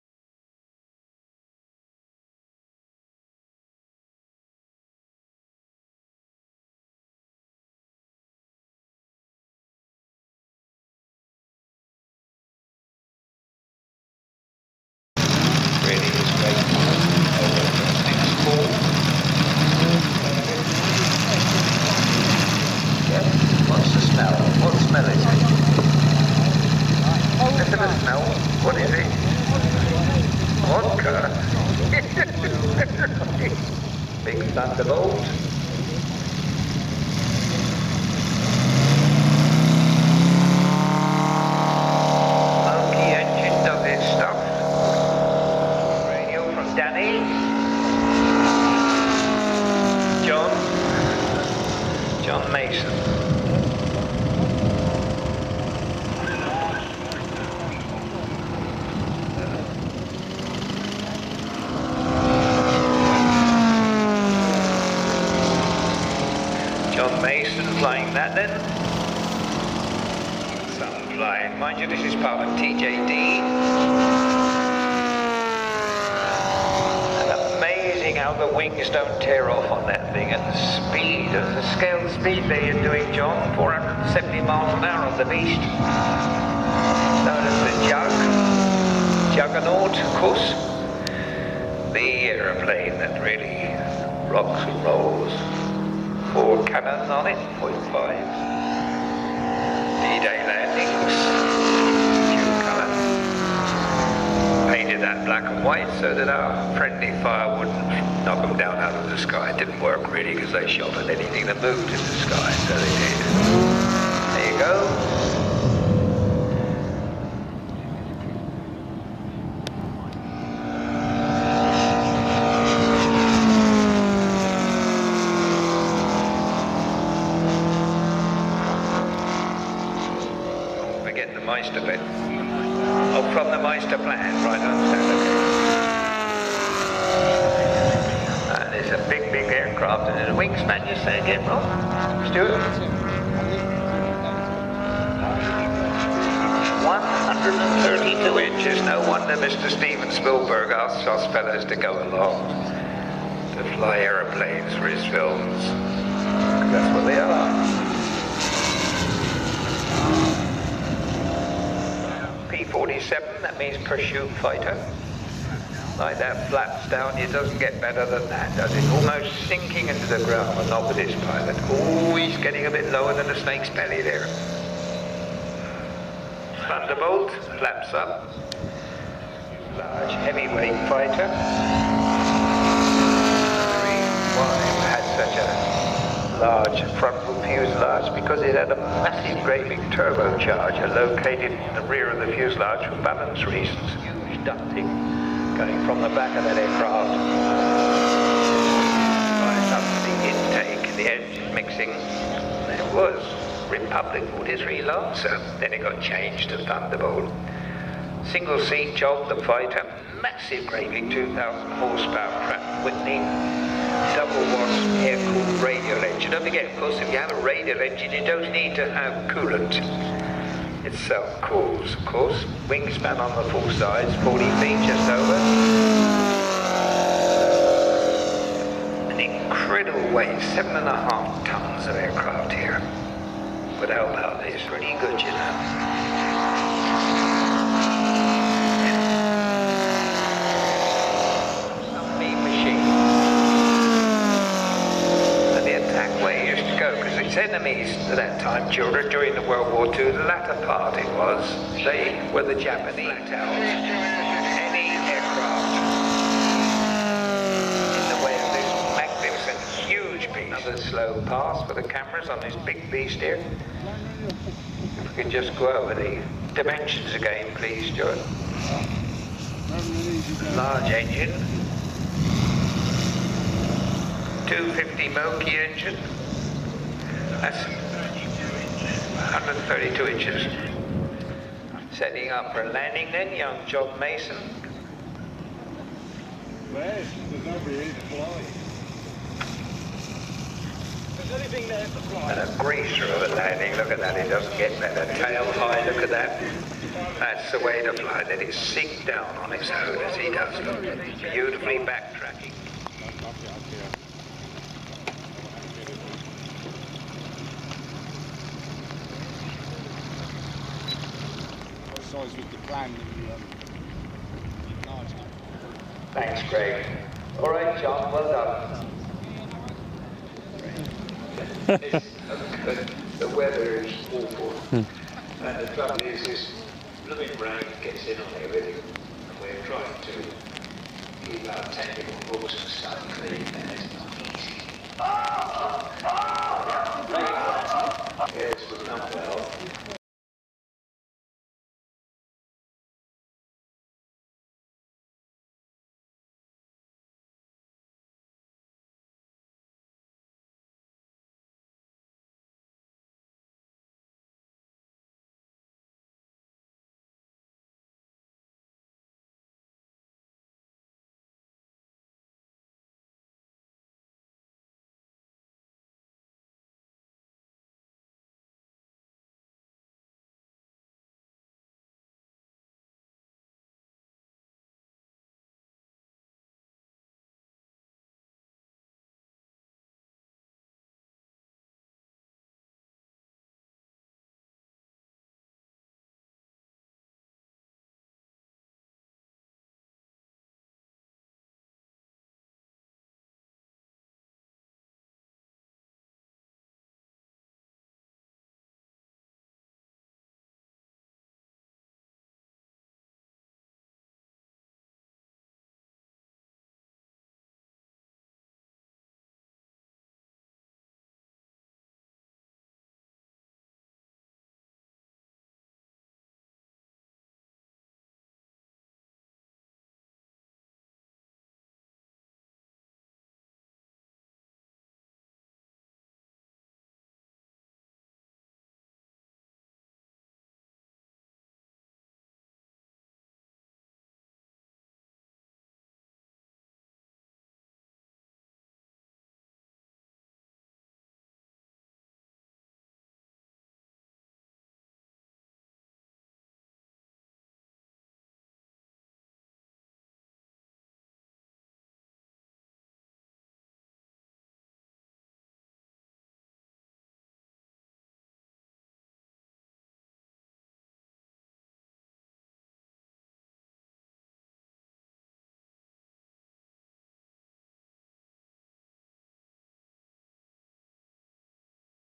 ② GIANT 1/4 SCALE RC P-47 - MOKI 250cc - JOHN MASON AT WINGS & WHEELS RC MODEL AIRCRAFT SHOW - 2014
THE P47 HAS A WING SPAN OF 132" AND WAS ENLARGED FROM THE MEISTER PLAN,
SHE WEIGHS 38 KG`S AND IS POWERED BY A MOKI 250cc 5 CYLINDER RADIAL FOUR STROKE PETROL ENGINE,
SHE WAS BUILT BY NIGEL`I ANSEN AND IS OWNED AND FLOW BY JOHN MASON
FROM TJD MODELS UK.

FILMED OVER THE WEEKEND OF JUNE 28th & 29th - 2014 AT THE WINGS & WHEELS MODEL SPECTACULAR AT NORTHWEALD AIRFIELD - EPPING - ESSEX - CM16 6AR ,
TURN OFF AT JUNCTION 7 ON THE M11,